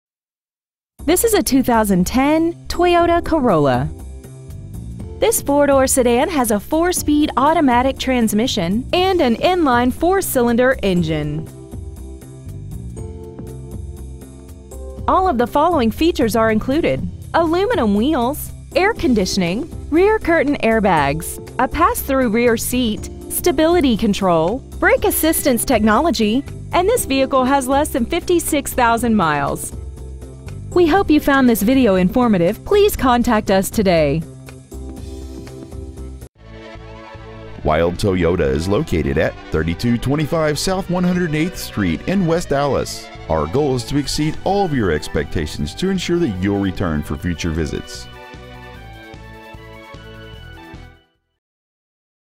Preowned 2010 Toyota Corolla Milwaukee WI
We have been honored to serve the West Allis WI area , we promise that your experience at our dealership will exceed your expectations ! Year : 2010 Make : Toyota Model : Corolla Engine : 4 Cylinder Engine Trans . : 4-Speed A/T Exterior : Classic Silver Metallic Miles : 55,694 Interior : Ash Stock : U66746 Wilde Toyota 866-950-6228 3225 South 108th St. West Allis , WI 53227